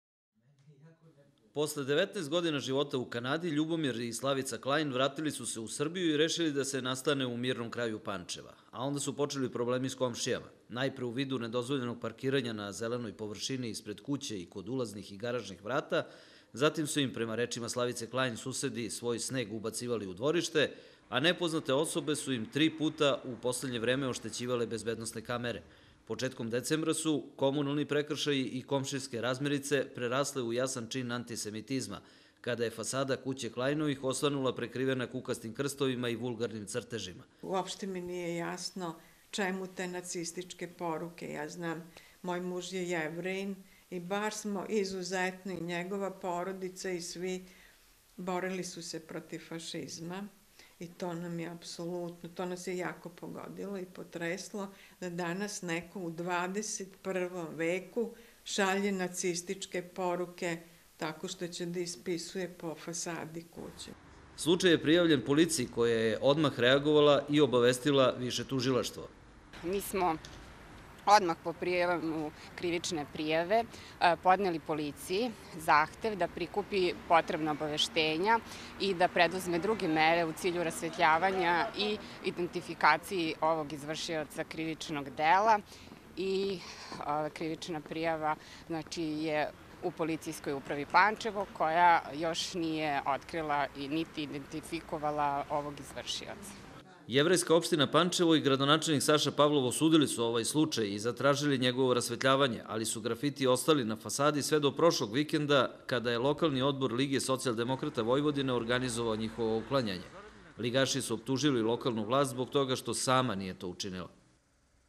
PA: Antisemitski grafiti na kući Klajnovih
Porodica Klajn koja se pre desetak meseci nakon dve decenije vratila iz Kanade i naselila u Pančevu sve vreme je doživljavala različite neprijatnosti, od pojedinih komšija da bi se sve na kraju eskaliralo u činu antisemitizma kojimse sada bave istražni organi.